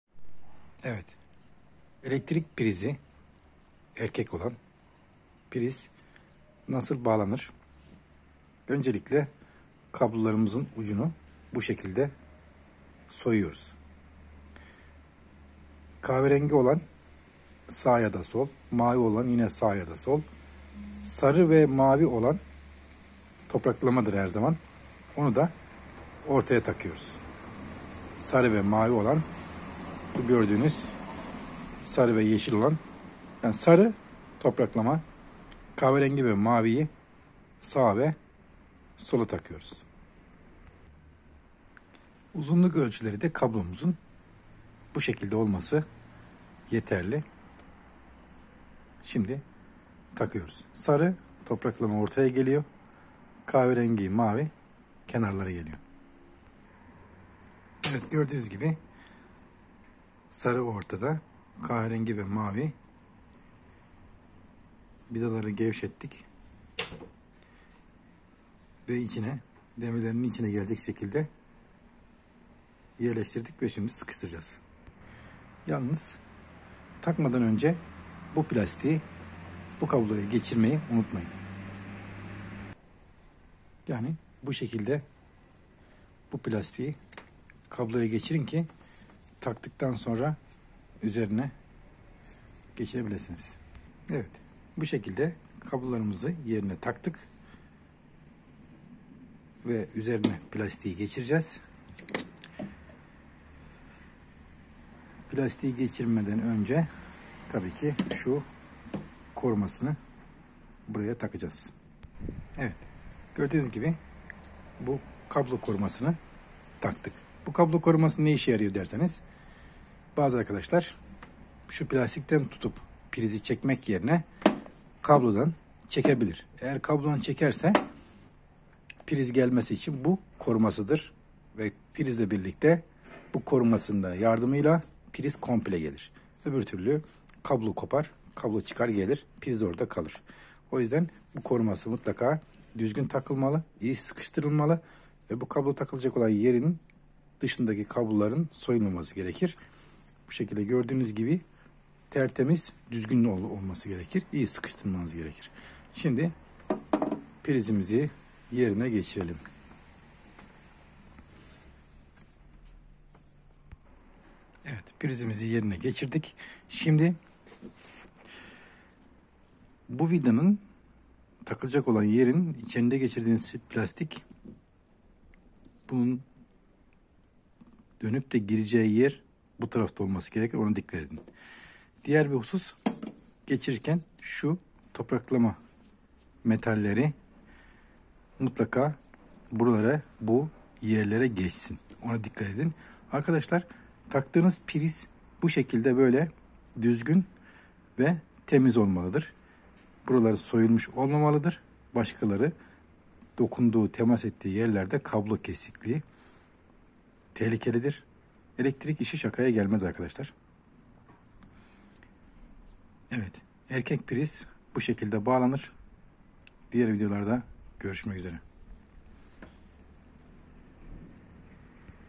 Priz nasıl takılır bağlanır şofben termosifon için
Priz nasıl takılır bağlanır değiştirilir yapılır sarı kablo topraklama olur her zaman kahverengi ve mavi kablo elektrik demektir faz biri şofben priz ile birlikte çalışır mı çalışır sorun olmaz ama kablo kaliteli ve kalın olmalıdır, şofben termosifon için